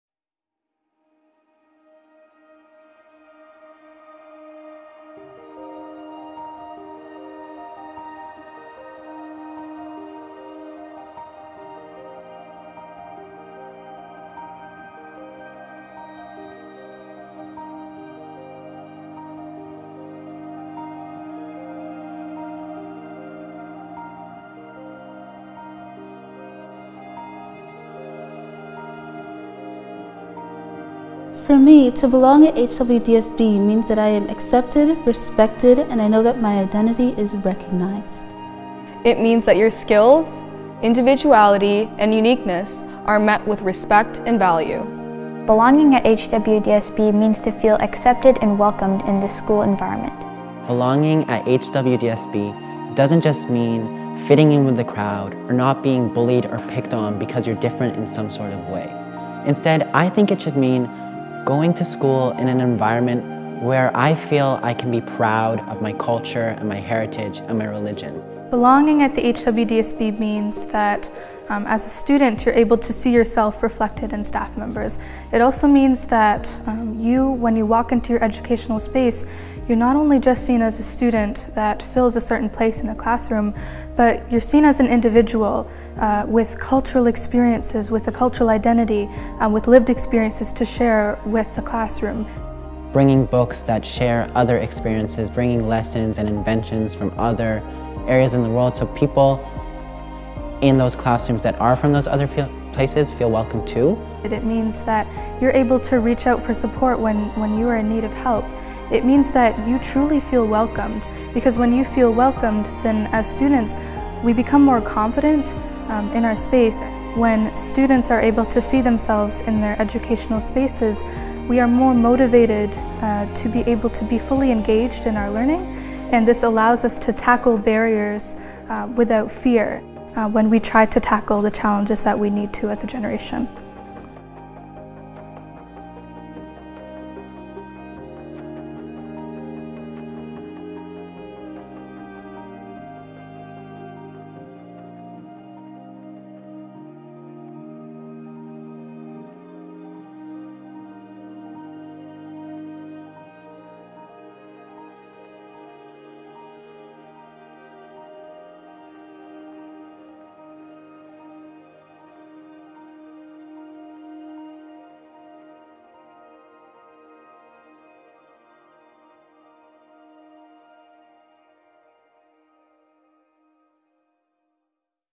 We Belong at HWDSB
We are co-creating equitable, welcoming and supportive environments where students and staff reflect our community's diversity and feel like they belong.

What does it mean to belong at HWDSB? Here's what HWDSB students told us...

Join our staff team by visiting hwdsb.on.ca/careers